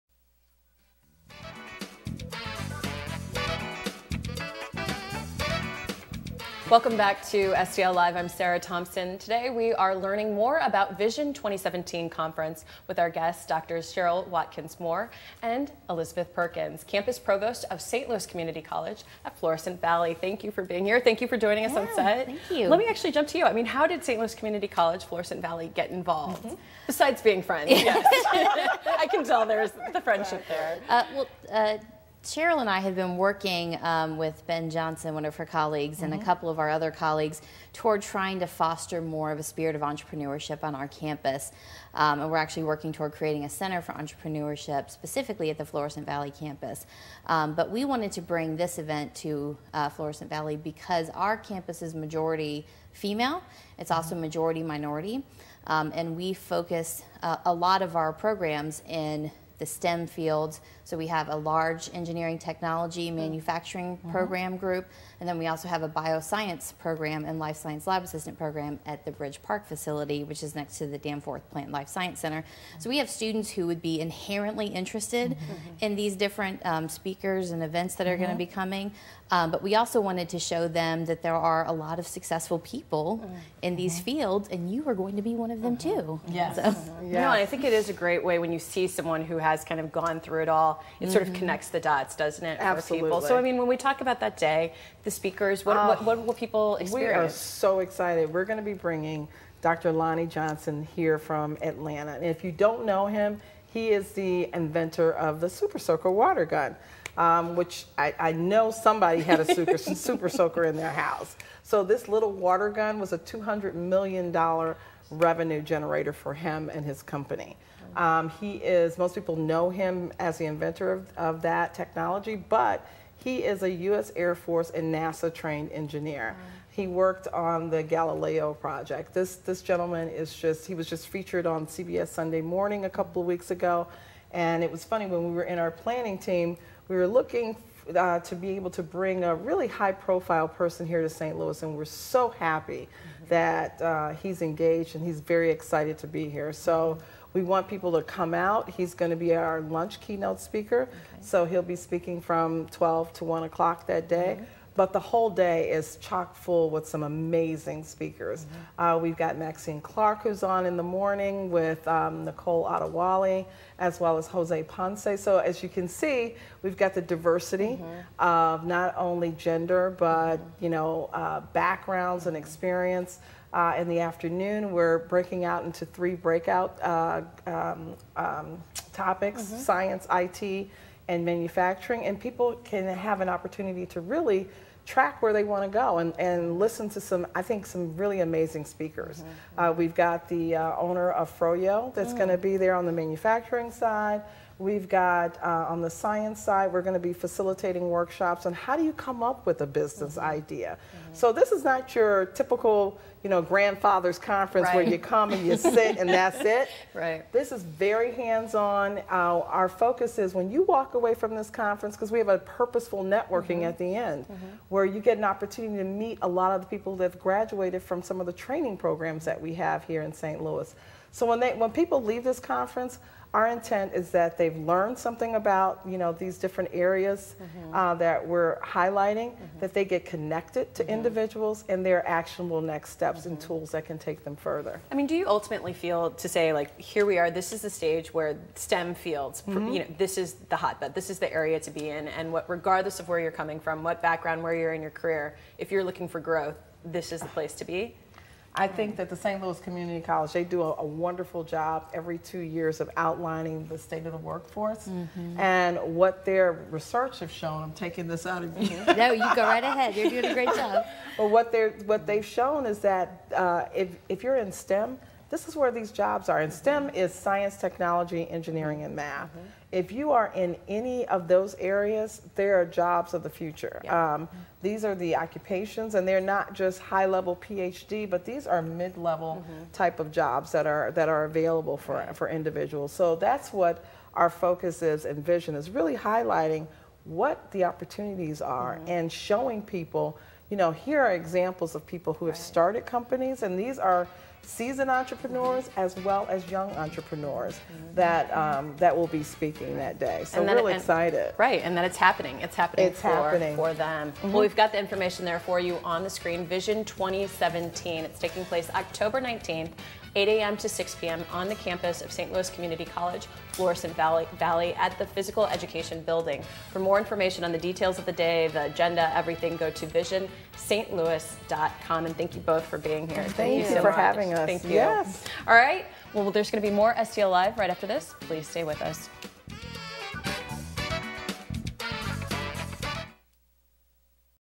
STL LIVE VISION St. Louis - 2 of 2
STL LIVE - VISION St. Louis
HOST: Sarah Thompson
SHOW DATE: October 10, 2017

VISION 2017 is symposium aimed at building St. Louis’ STEM small businesses into an inclusive and diverse community. Here with more are doctors Cheryl Watkins-Moore of BioSTL and Christy Maxfield, Program Consultant for the Center for Emerging Technologies…

Guest:  Dr. Cheryl Watkins-Moore/ BioSTL
Guest:  Dr. Elizabeth Perkins/ Campus Provost, STL Community College - Flo Valley
Guest:  Christy Maxfield/Program Consultant for the Center for Emerging Technologies

VISION/ Small Business Innovation & Inclusion Symposium/ October 19, 8 am - 6 pm/ St. Louis Community College - Florissant Valley/ visionstlouis.com

Vision 2017 is a small business symposium dedicated to building an inclusive and diverse STEM community in St. Louis. The annual conference focuses on those starting businesses in science, IT and manufacturing and will have interactive learning opportunities, a speaker and networking reception. VISION 2017 takes place at St. Louis Community College - Florissant Valley on October 19th from 8 am until 6 pm.